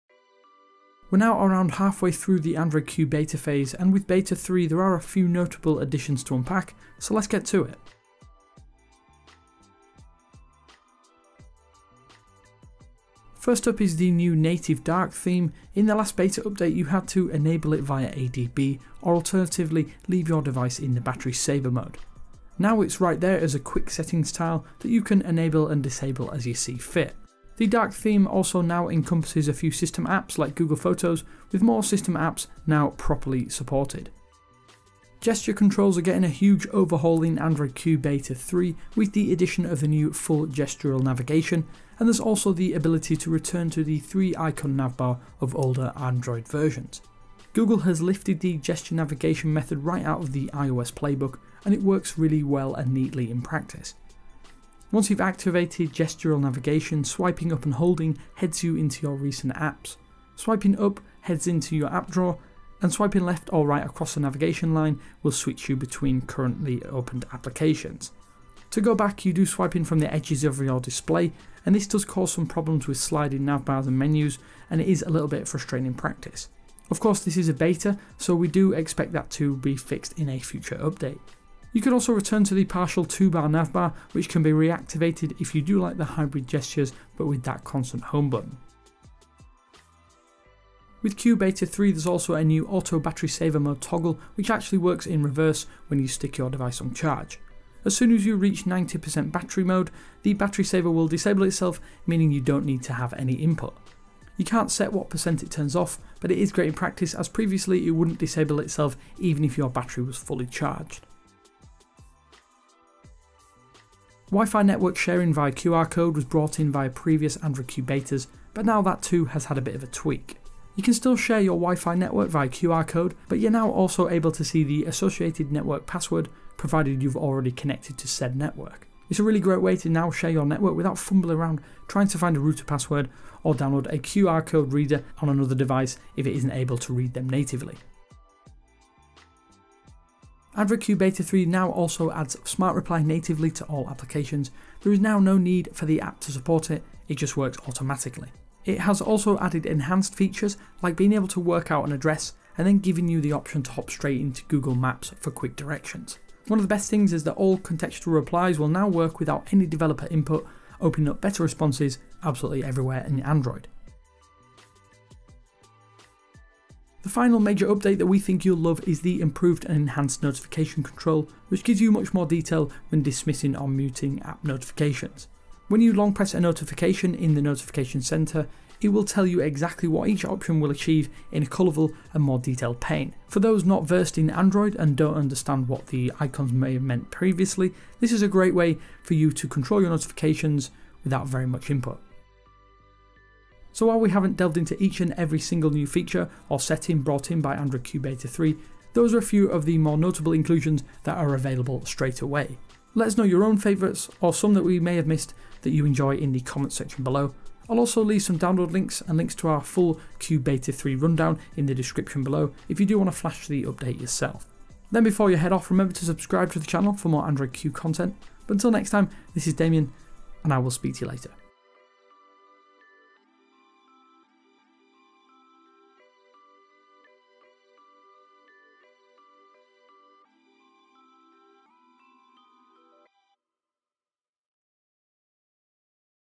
Android Q Beta 3: Top 7 new features!
All of the most notable features as part of Android Q Beta 3.

My 🎥🎙gear:
Panasonic Lumix GH5

Rode NTG-3